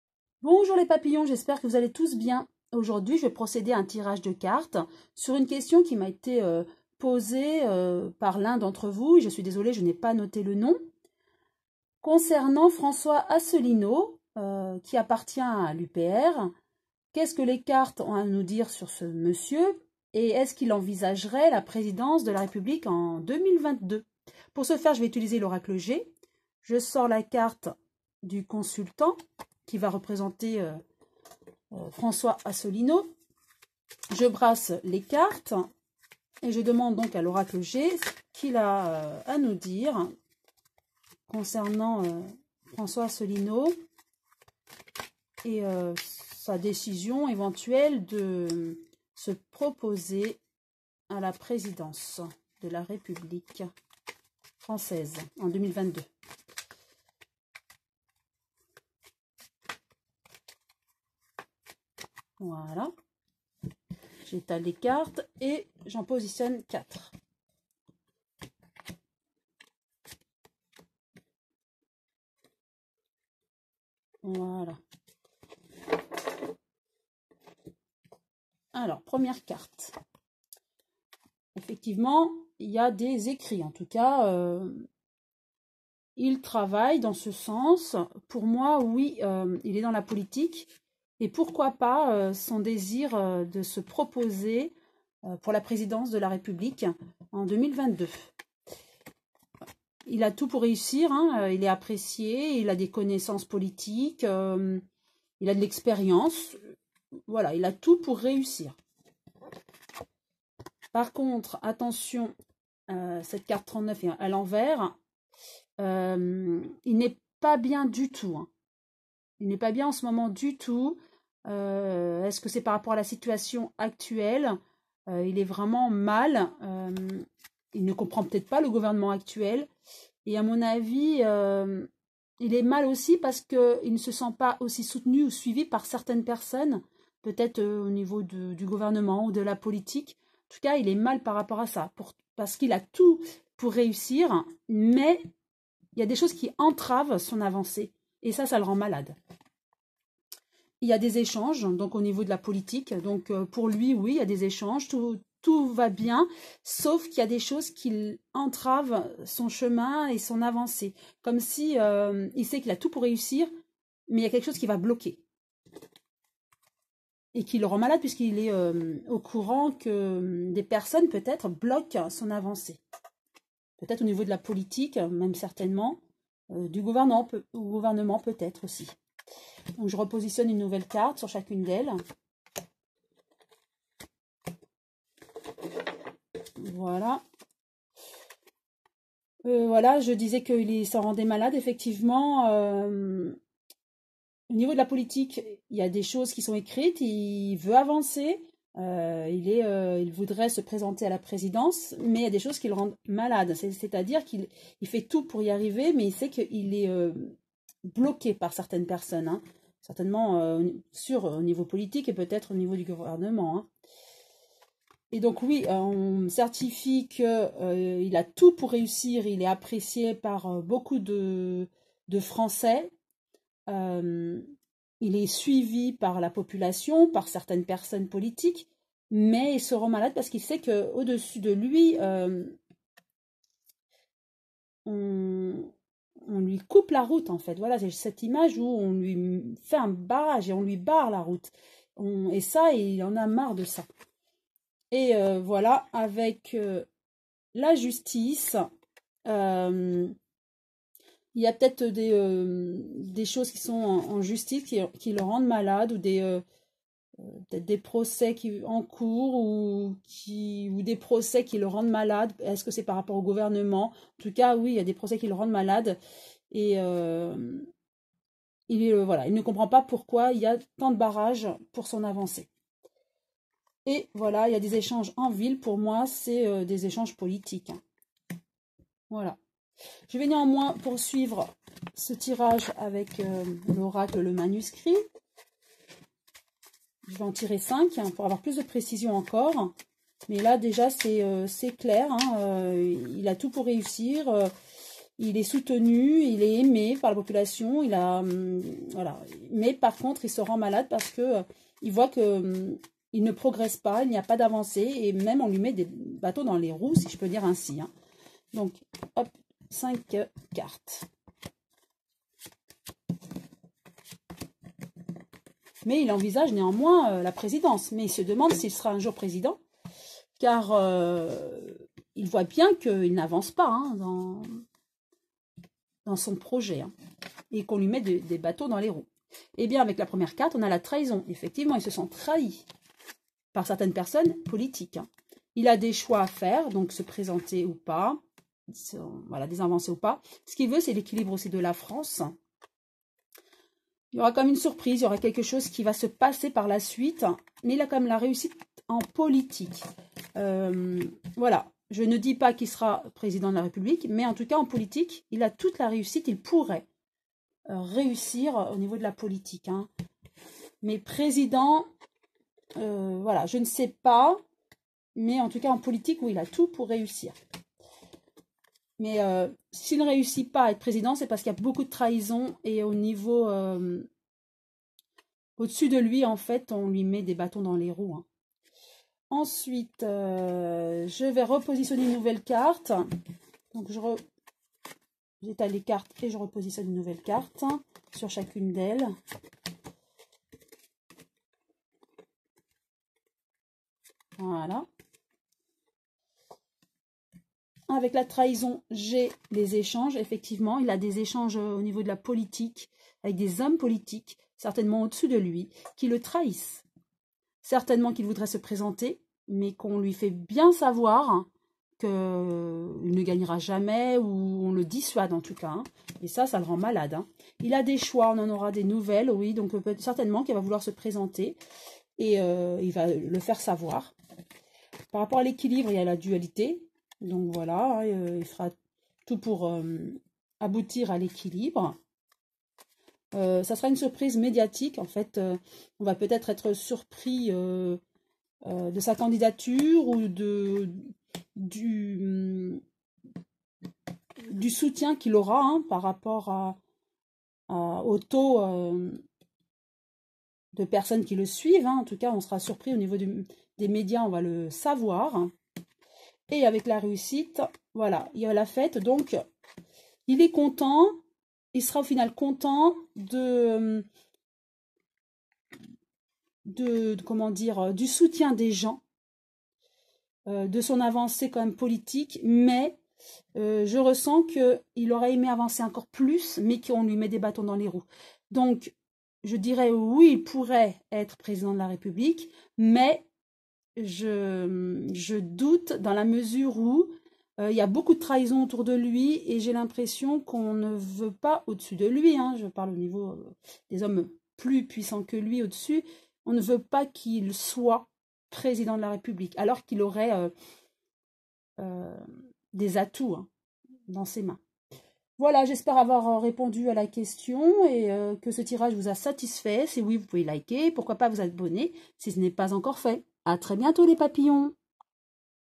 Tirage ~ Que dire sur François ASSELINEAU ?
Bonjour les Papillons,
Je suis heureuse de vous avoir informés sur ce que révèlent les cartes concernant François ASSELINEAU.

Vérifiez bien que vous serez avertis de toutes les notifications en recliquant sur la cloche et en notant TOUTES au niveau des OPTIONS.